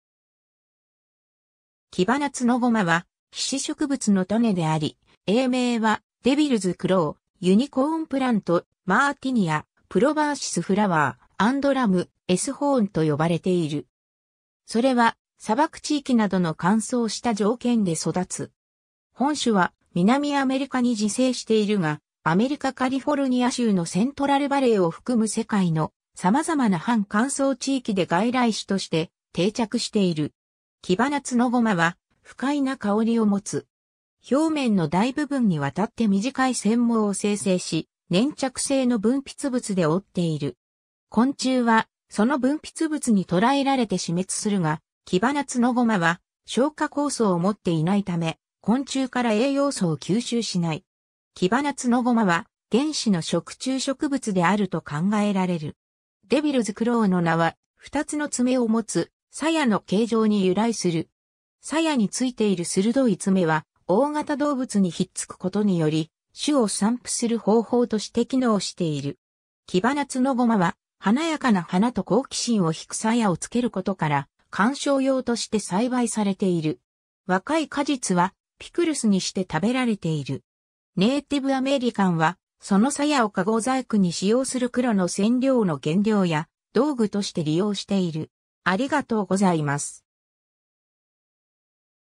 キバナツノゴマ
キバナツノゴマ（学名 : Ibicella lutea）は、被子植物の種であり、英名は、devil's claw（悪魔の爪）, unicorn plant（ユニコーンの植物）, martynia, proboscis flower（テングの花）, and ram's hornと呼ばれている。
それは砂漠地域などの乾燥した条件で育つ。
本種は、南アメリカに自生しているが、アメリカカリフォルニア州のセントラル・バレーを含む世界の様々な半乾燥地域で外来種として定着している。
キバナツノゴマは、不快な香りを持つ。
表面の大部分にわたって短い腺毛を生成し、粘着性の分泌物で覆っている。
昆虫は、その分泌物に捕らえられて死滅するが、キバナツノゴマは消化酵素を持っていないため、昆虫から栄養素を吸収しない。
キバナツノゴマは原始の食虫植物（虫を捕えるが食虫植物に成りきれていない植物）であると考えられる。
devil's claw（悪魔の爪）の名は、2つの爪を持つさやの形状に由来する。
サヤについている鋭い爪は、大型動物に引っ付くことにより、種を散布する方法として機能している。
 キバナツノゴマは、華やかな花と好奇心を引くさやをつけることから観賞用として栽培されている。
 若い果実は、ピクルスにして食べられている。
ネイティブ・アメリカンは、そのさやをかご細工に使用する黒の染料の原料や道具として利用している。